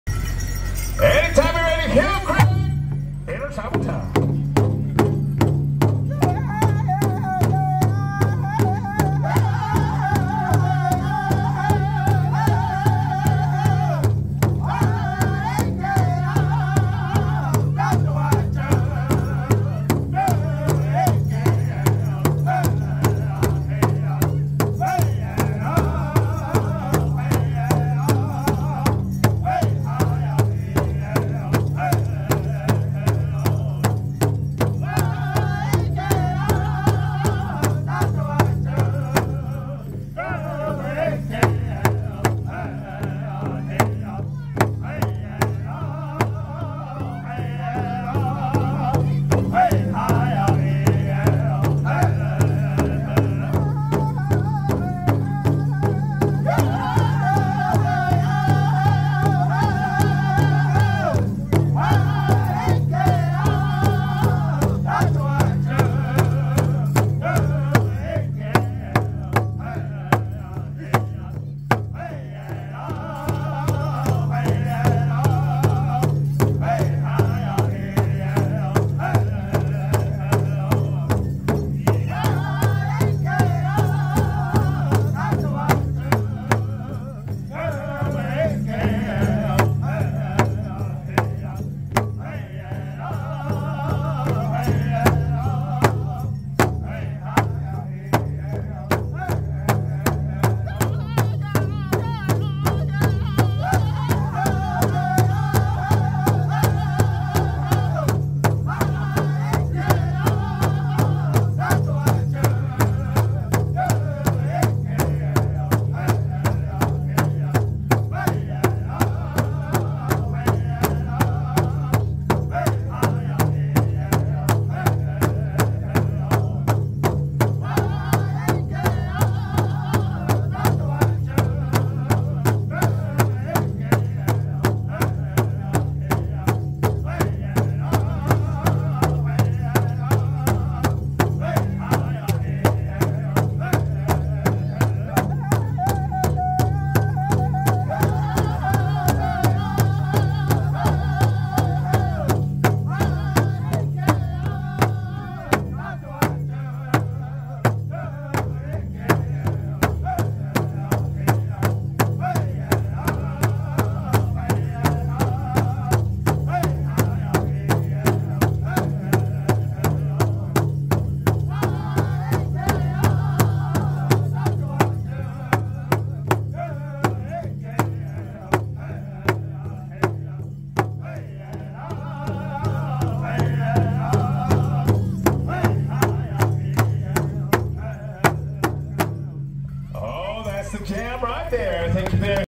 Hill Creek (Contest Song) l Heber Powwow 2023
Hill Creek (Contest Song) during the Intermountain Champions Powwow, Heber Valley, Utah, June 3, 2023.